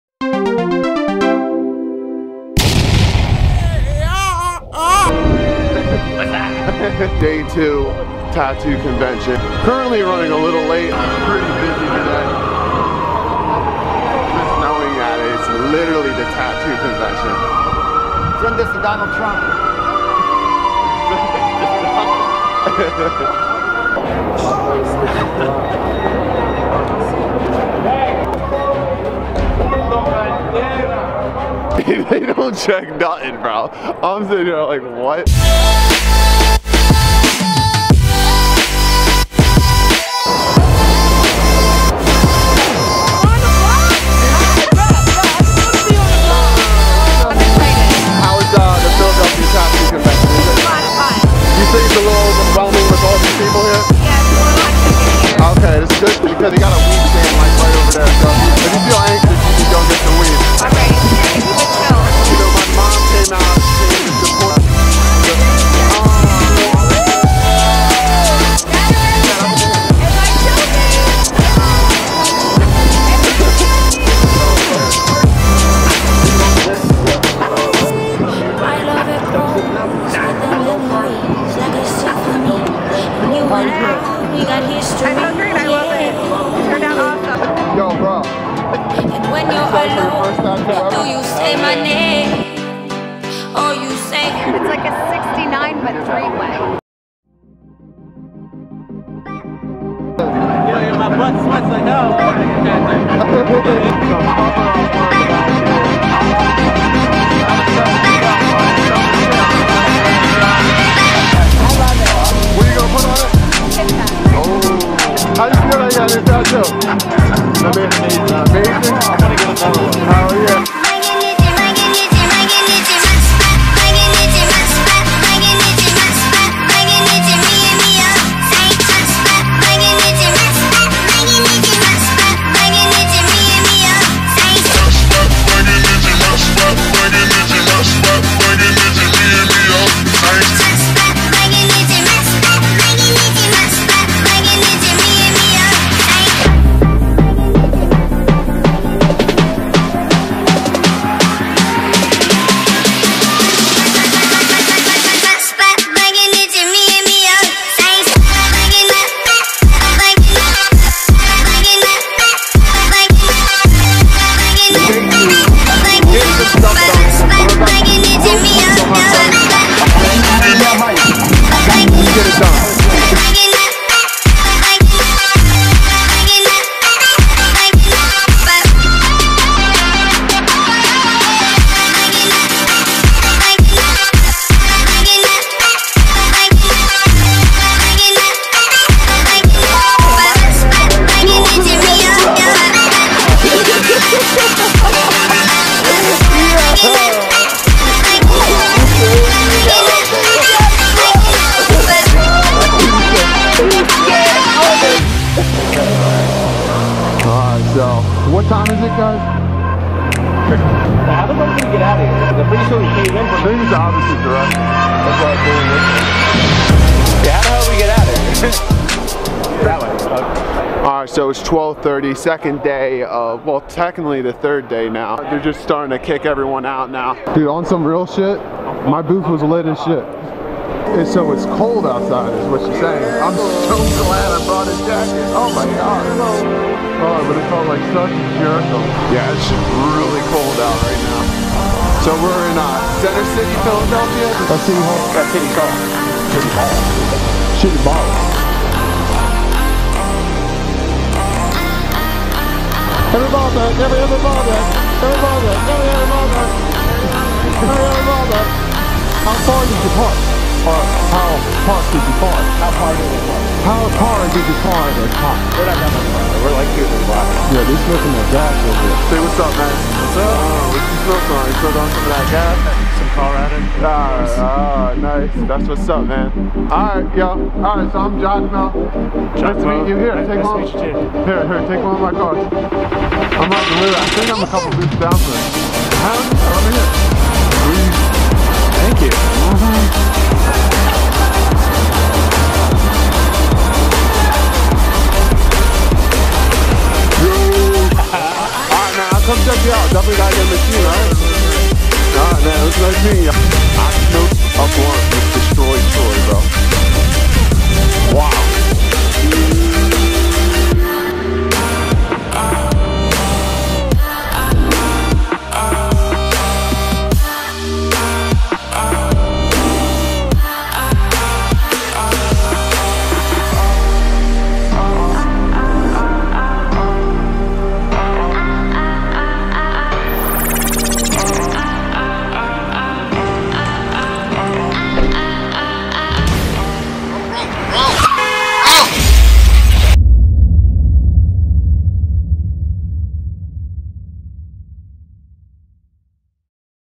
THE CRAZIEST PHILADELPHIA TATTOO CONVENTION SO FAR 2019 TITTATCONTENT EP 9 (VLOG) PART 2
Tattoo / Microblading artist Giacomo Giovinetti takes you on the mythical adventures of the second day of the Philadelphia tattoo convention running into trouble meeting new friends and gives you a behind the scenes look as an artist life style as well as his mission of becoming the best in his craft.
Sit back and enjoy the vlog of a life time laughs, smiles, and get ready to be inspired(part 2)